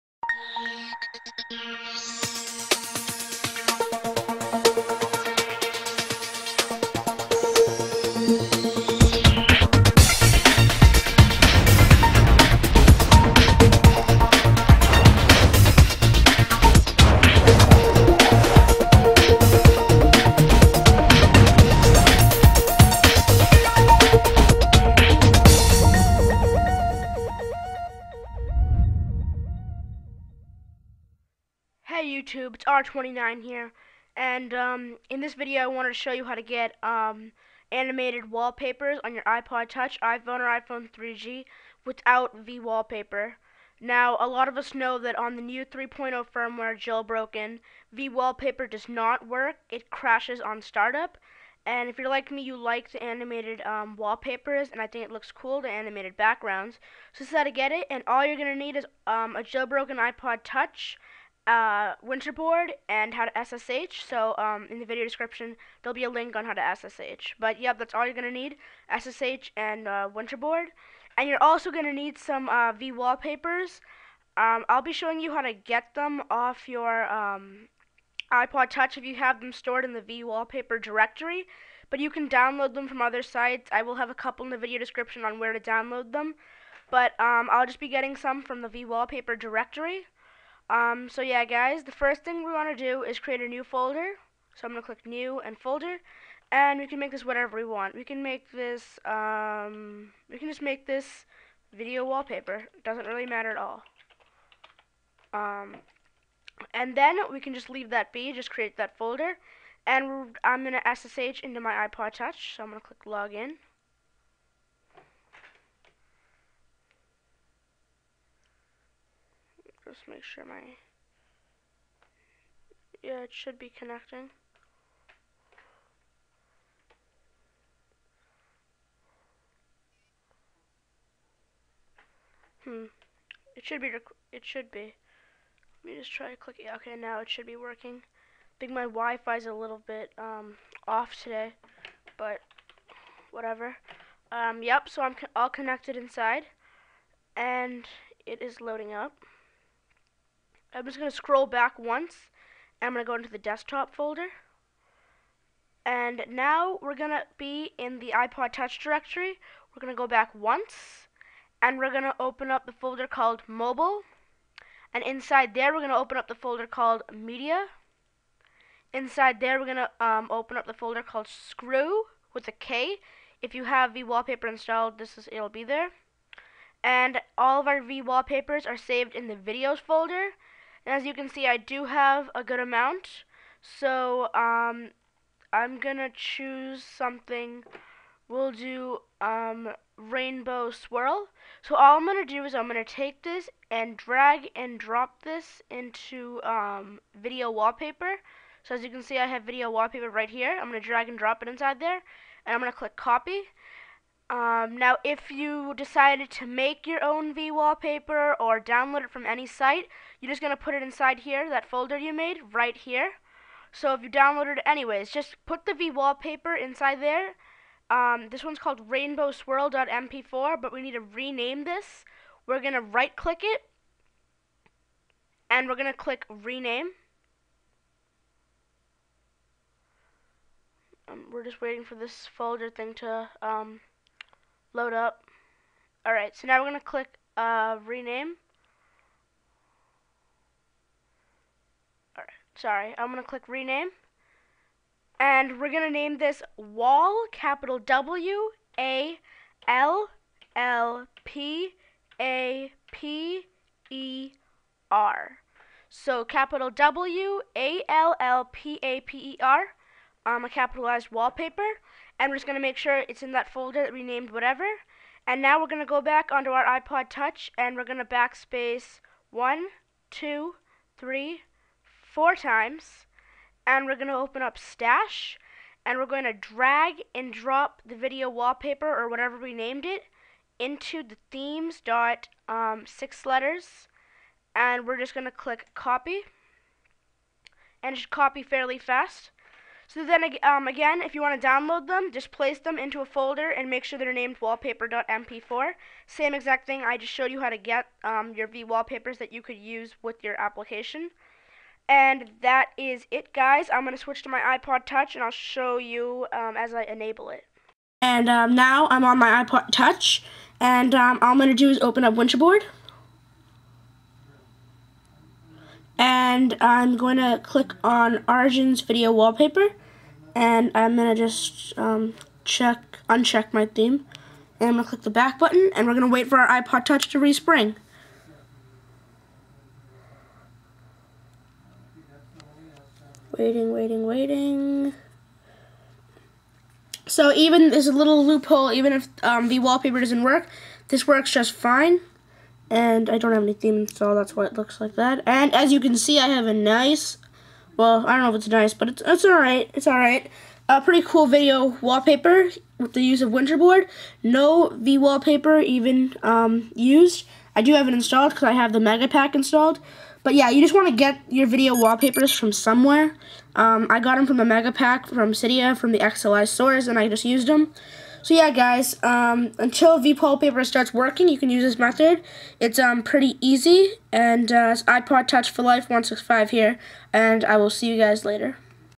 How To Get Animated Wallpapers on iPod Touch, iPhone, iPhone 3G on 3.0 NO VWALLPAPER NEEDED!!!
README!!!

Hey guys. As we all know, vWallpaper crashes, so here's who to do it with Winterboard.  It works on 3.0, but you can do this on 2.x.

OPT. To get the videos that you had in the vWallpaper app, navigate here:
/var/mobile/Media/Skrew/Videos
Copy any of the ones you like onto your computer.

1) Get a video wallpaper from somewhere. You can use the one you got from the vWallpaper directory explained above, or you can download them from these sites:

Note: You need to be registered at modmyi.com to download them form the second link.

2) Rename the video : Wallpaper.mp4

3) Put that renamed video into a folder that you canname whatever you want.
i.e. Arjun's Video Wallpaper

4) SSH into your device.

5) Backspace once, and click on stash.
/var/stash/

6) Drag-and-drop (copy) that folder that you named whatever you wanted with the video inside into
/var/stash/Themes.xxxxxx

7) Go into Winterboard and enable the theme. Click the home button and wait for it to respring!